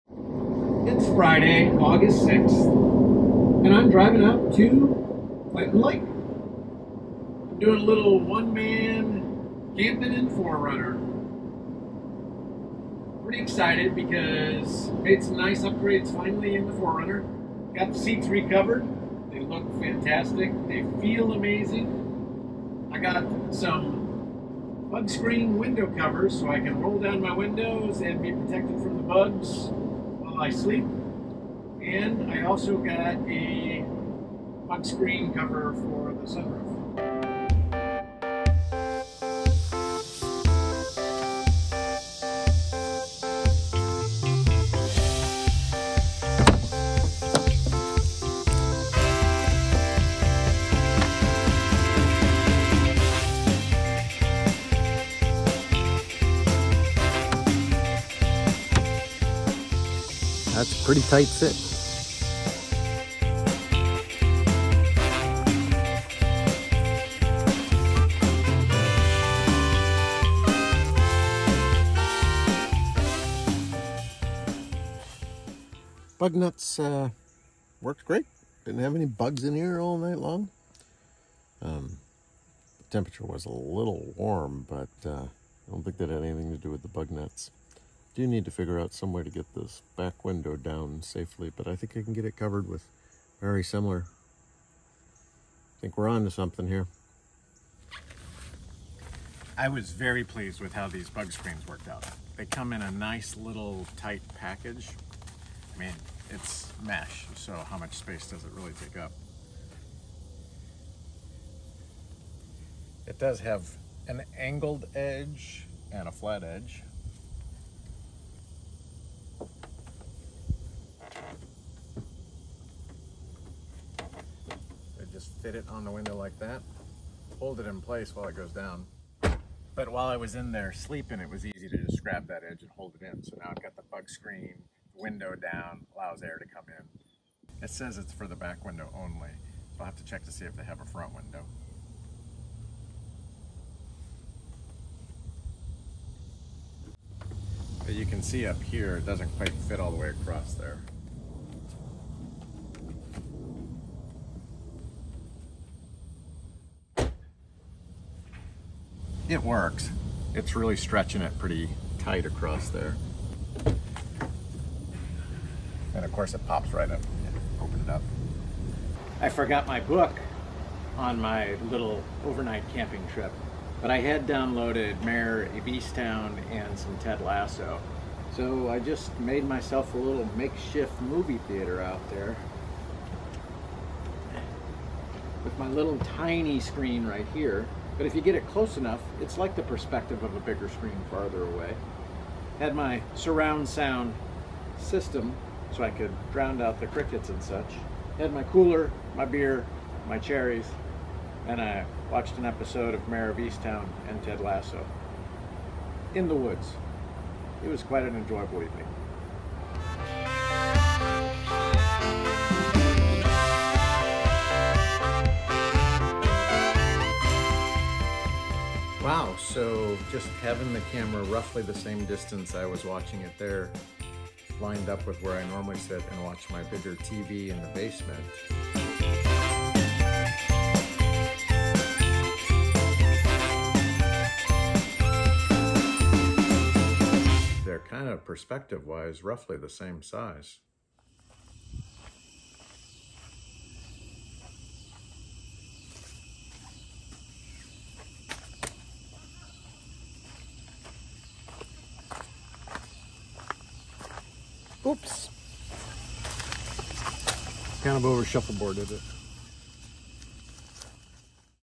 CAR CAMPING with my NEW BUG SCREENS
The 4Runner as my camping vehicle is coming along. Plus I make a theater set up at my camp site.

I actually purchased both through Amazon.